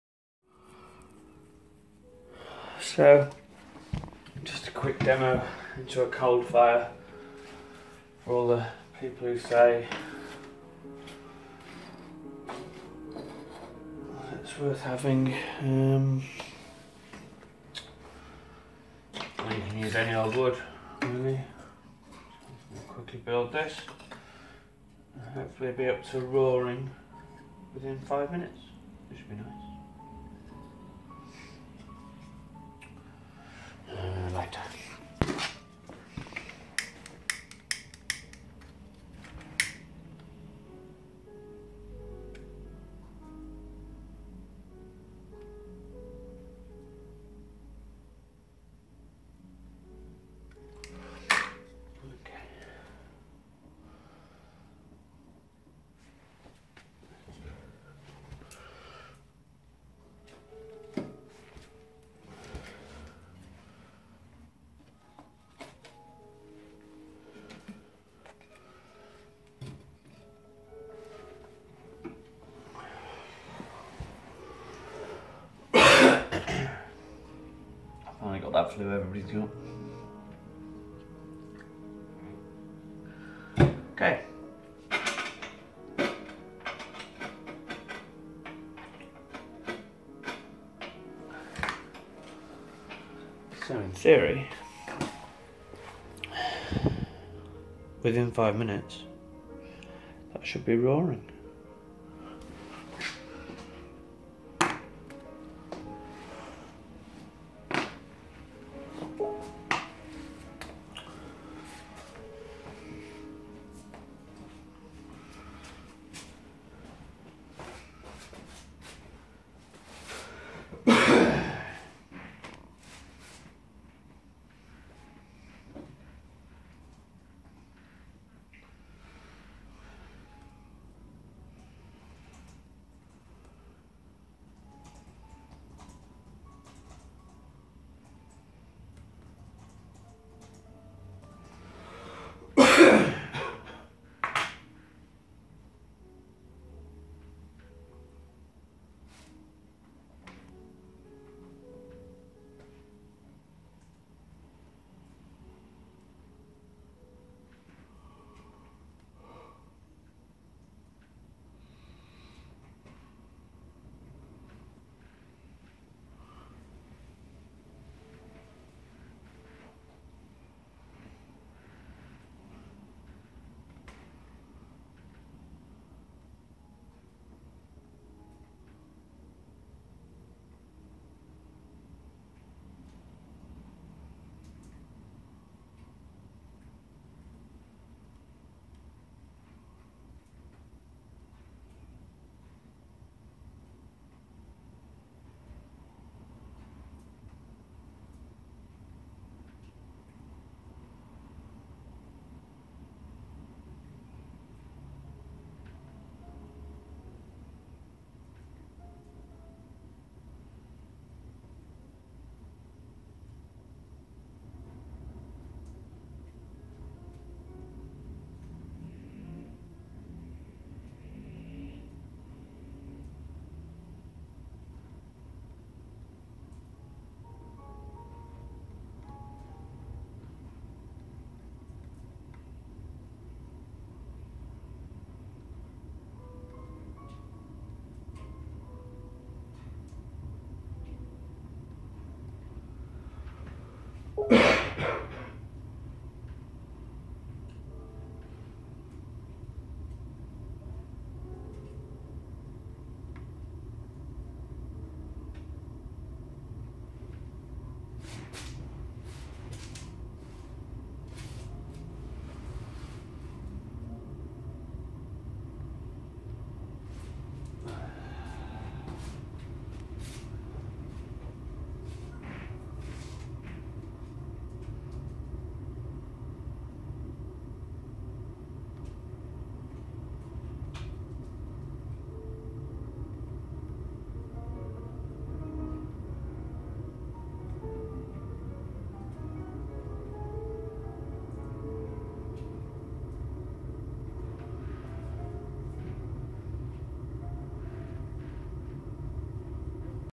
Kiln dried from CertainlyWood 0-5min
It is worth the extra money I think. Absolutely none of that 'has the fire gone out' nonsense. The self cleaning window can kick in almost straight away as it gets to 700 degrees almost instantly.